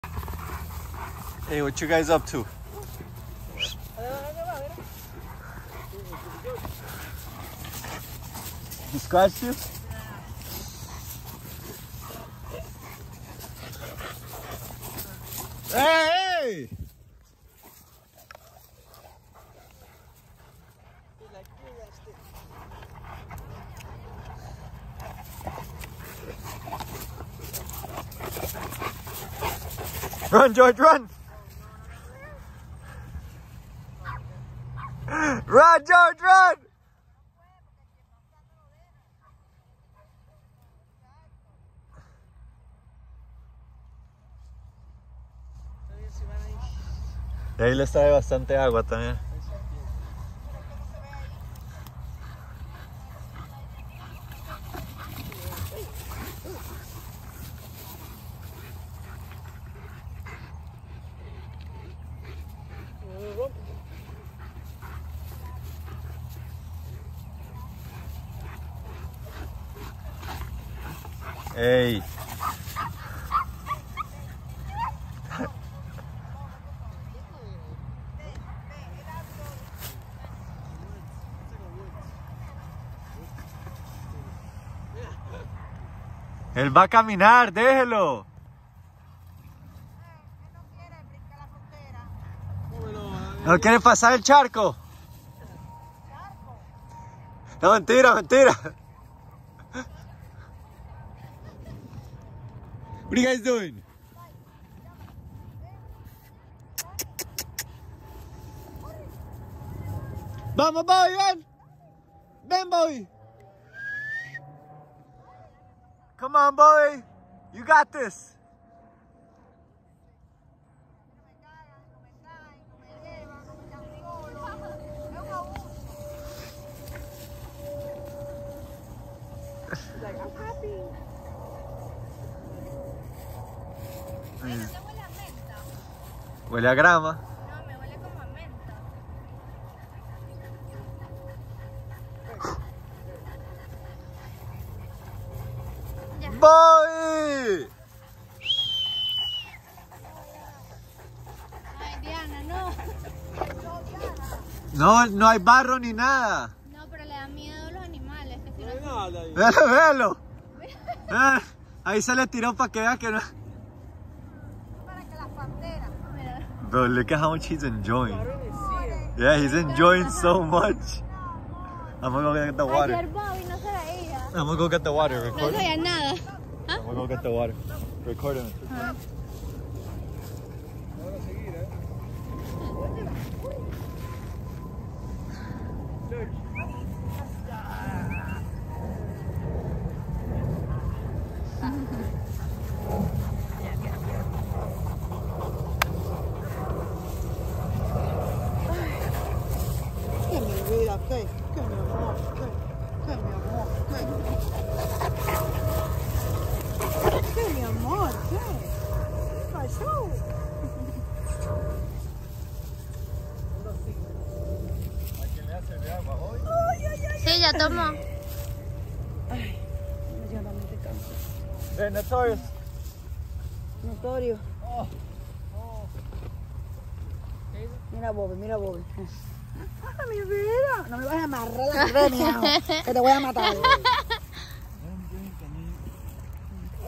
CANE CORSO IN THEIR NATURAL HABITAT…
They love coming to this area, literally play hide and seek with the tall grass around them 😂😂